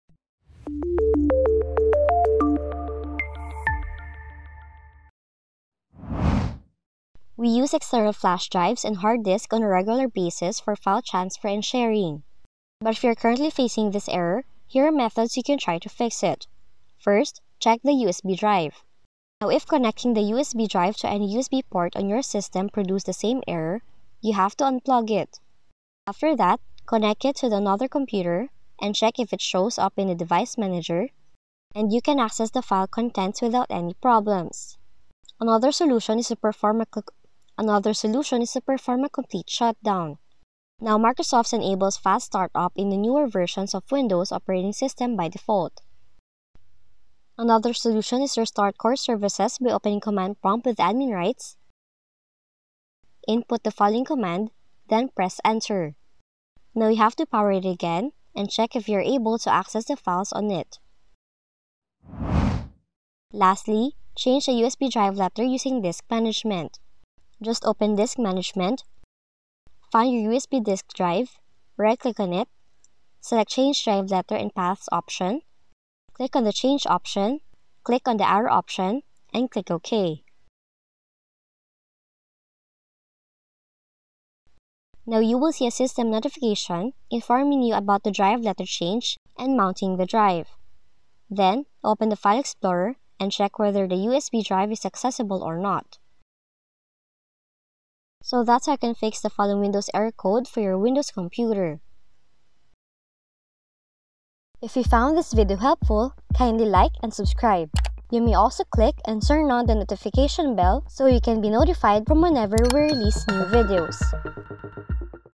FIX Windows Error: “You Need to Format the Disk in Drive Before You Can Use It”
Discover how to fix the Windows error, “You need to format the disk in drive before you can use it” with this comprehensive guide. Learn step-by-step methods to address and resolve disk formatting issues without losing data.